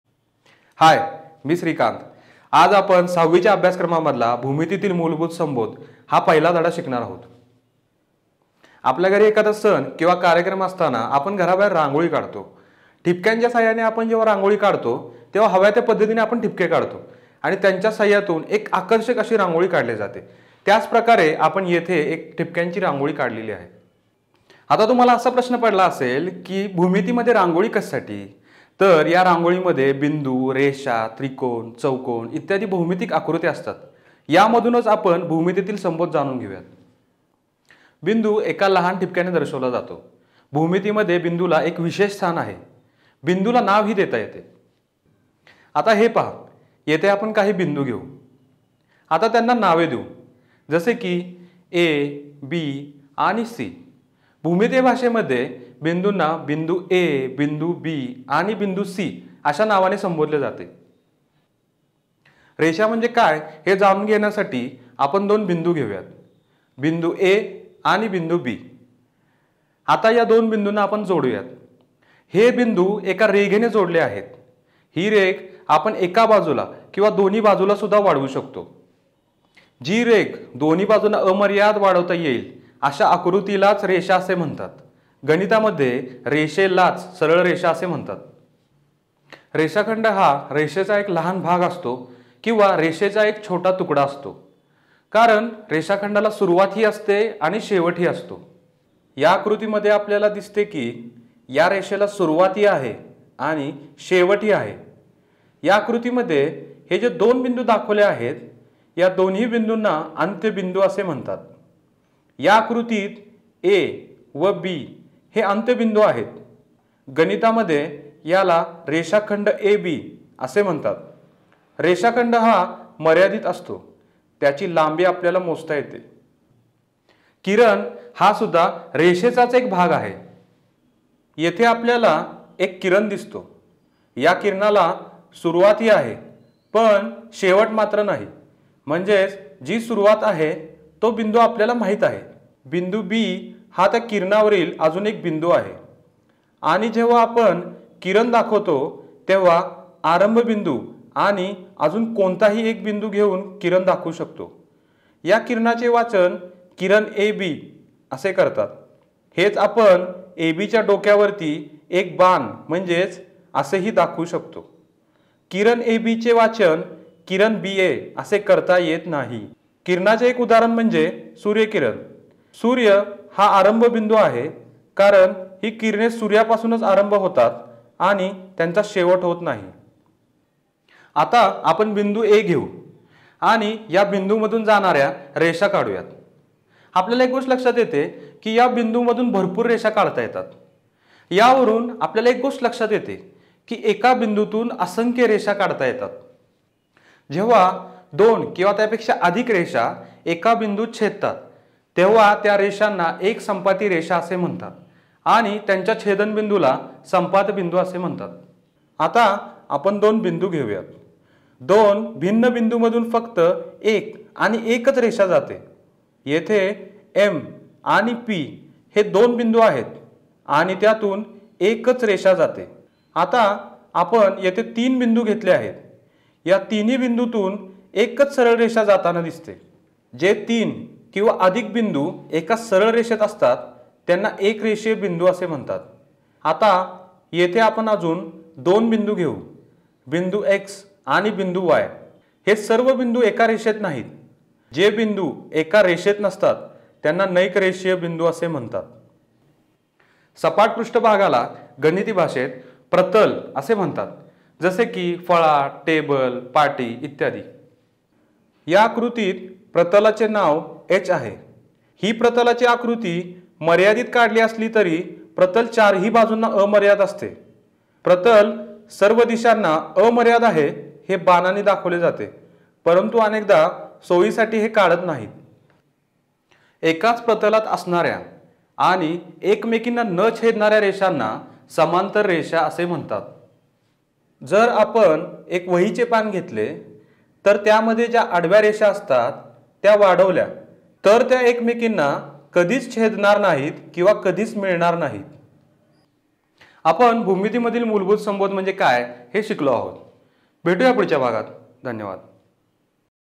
6th Standard Maths Chapter 1 - Basic Concepts in Geometry(भूमितीतील मूलभूत संबोध)
भूमितीतील मूलभूत संबोध.
इबलेंड एज्युकेशनने मराठी माध्यमिक विद्यार्थ्यांसाठी पहिला व्हिडिओ सादर केला. कृपया आपल्याला या व्हिडिओबद्दल काय वाटते याबद्दल Comment Section मध्ये आम्हाला सांगा. आणि शेअर करायला आणि सबस्क्राईब करायला विसरू नका.